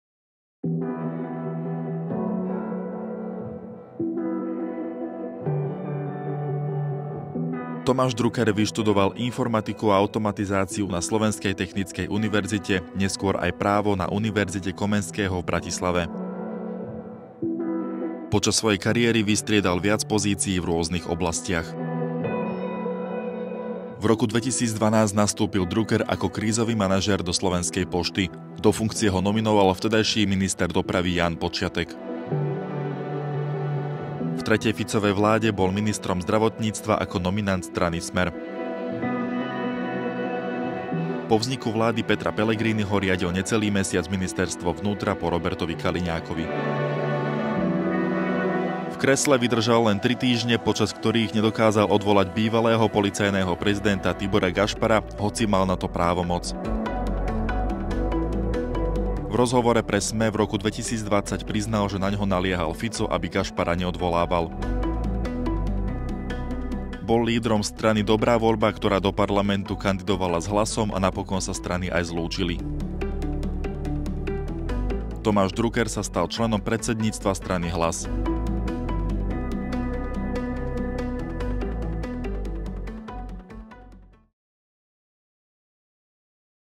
Kto je minister školstva Tomáš Drucker?
Vyštudoval informatiku a automatizáciu na Slovenskej technickej univerzite, neskôr aj právo na Univerzite Komenského v Bratislave.

Počas svojej kariéry vystriedal viac pozícií v rôznych oblastiach.

Bol lídrom strany Dobrá voľba, ktorá do parlamentu kandidovala s Hlasom a napokon sa strany aj zlúčili. Neskôr sa stal členom predsedníctva strany Hlas.

Pozrite si profil nového ministra školstva.